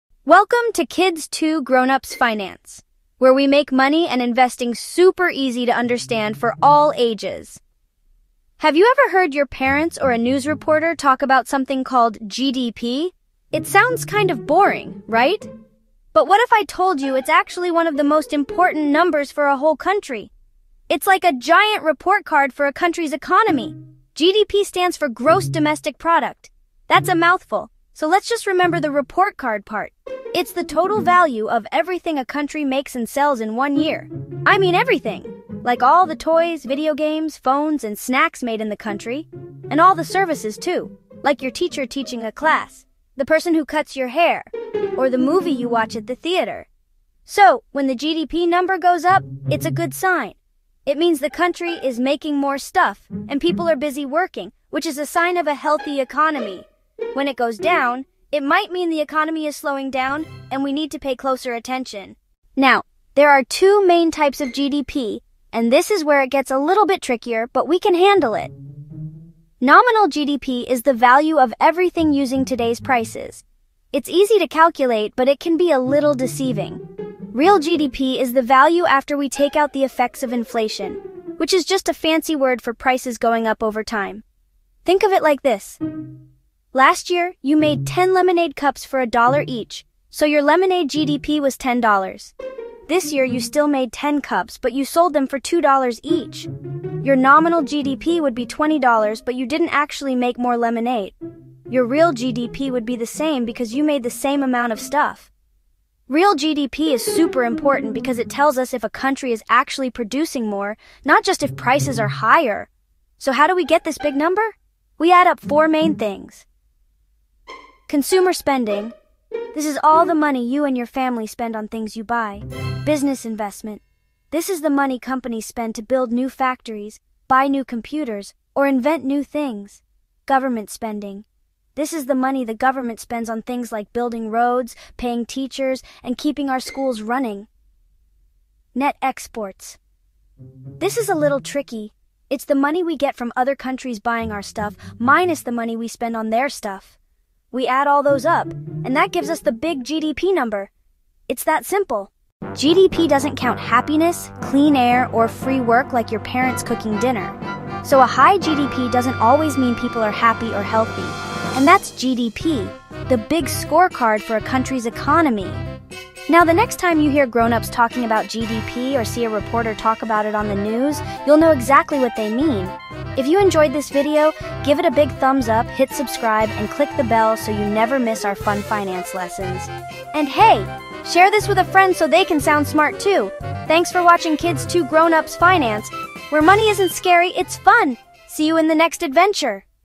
GDP Explained for Kids | Fun & Simple Guide to the Economy
What is GDP? Learn the fun and simple way! In this video, we explain Gross Domestic Product so kids and teens can understand how it measures a country’s economy. Using real-life examples, kid-friendly analogies, and easy explanations, we break down goods, services, types of GDP, and why it matters. Perfect for students, parents, and curious minds! GDP EconomyForKids KidsFinance LearnEconomics"